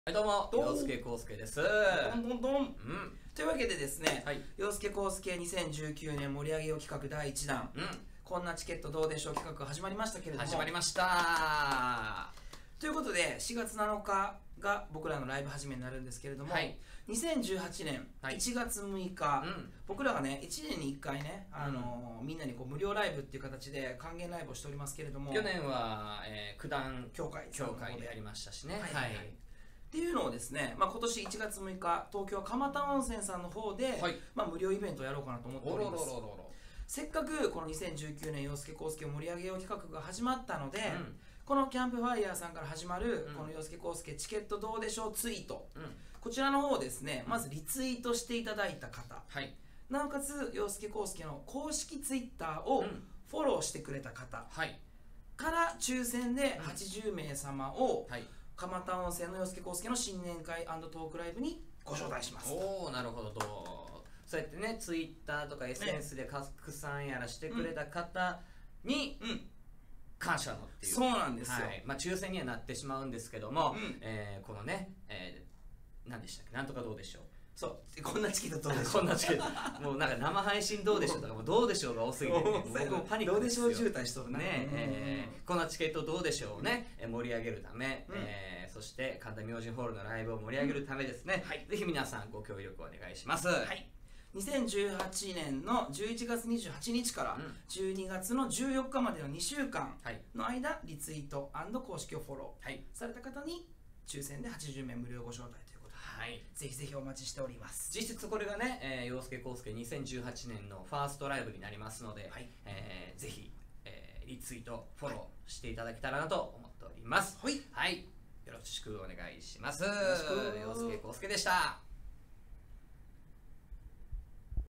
2019/1/6フリーライブご招待キャンペーン【ヨースケコースケ ファン感謝祭 2019】
【フリーライブご招待キャンペーン】
★ヨースケコースケ　ファン感謝祭 2019★
毎年恒例のヨースケコースケから皆様への感謝のご招待トーク＆ライブ！！
ヨースケコースケと一緒に新年を過ごしましょう！！
・2019年1月6日(日)蒲田温泉
東京都大田区蒲田本町2-23-2
・13:00開場 / 14:00開演
・80名ご招待(抽選/無料)

・応募方法
①+②をしていただいた方の中から抽選でご当選された方のみのご招待となります。
※当選発表：2018年12月19日(水)21:00～予定
※当選された片のみtwitterのDMにてご連絡いたします(整理番号有り)
※鍵アカウントは対象外とします

【ヨースケコースケのこんなチケットどうでしょう？？】YKF連続企画第一弾！の詳細はCAMPFIREのプロジェクトページでご確認ください。